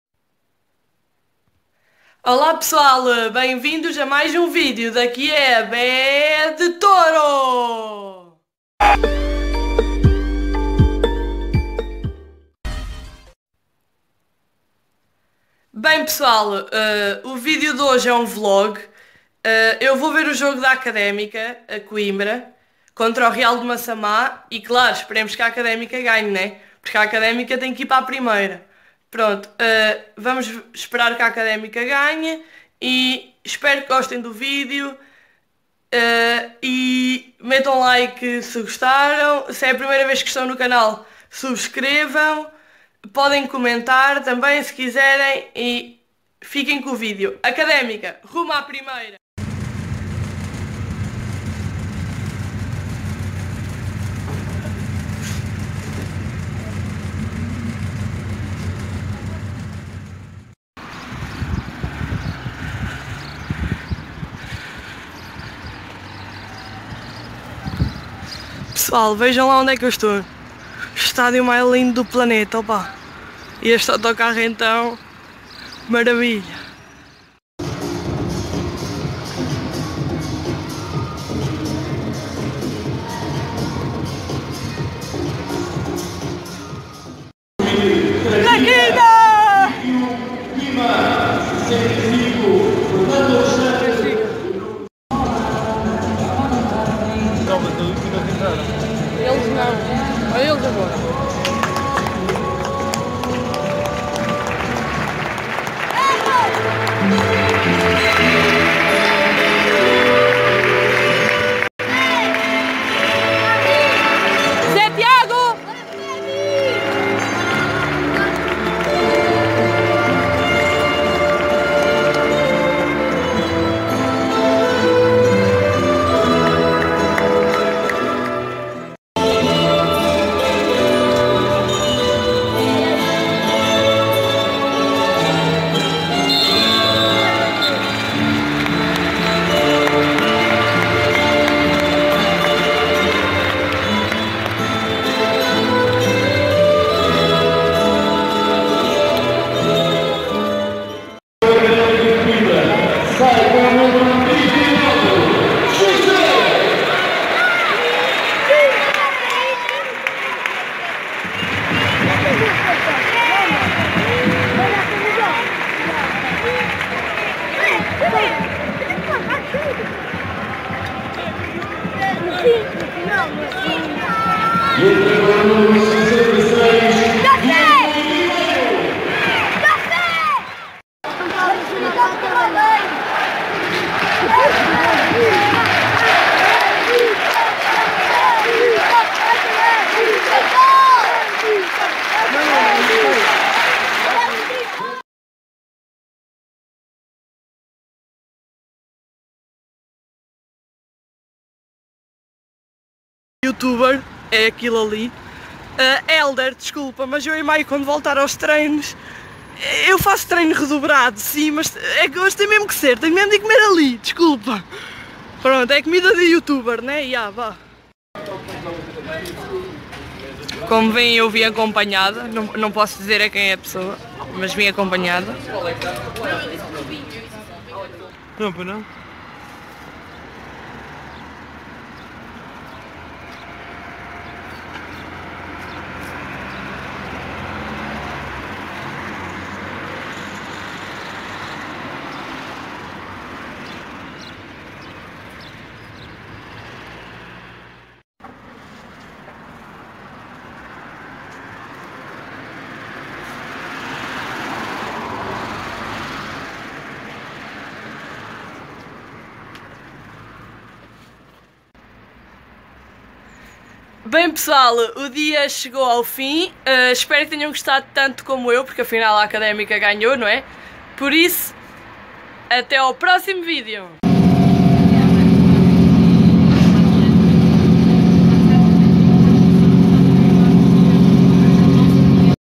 Fui ver a Académica e voltei a estar com os meus jogadores favoritos
Agradecimentos:

Académica OAF
João Traquina
Toze Marreco